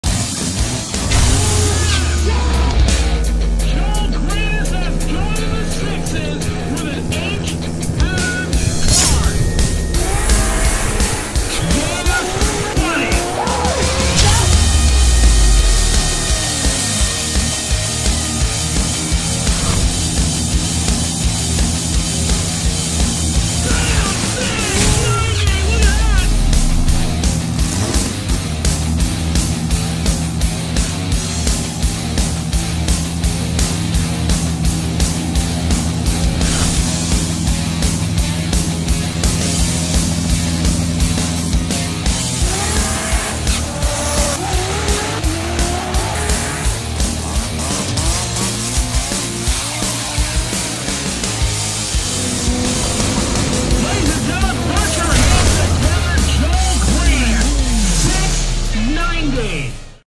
Airbrush SUPERSTAR David Reyes paints the Grannas Racing Supra (short preview)
This guy is AMAZING! David Reyes paints the grannas racing supra! Make sure you check out his work on Instagram!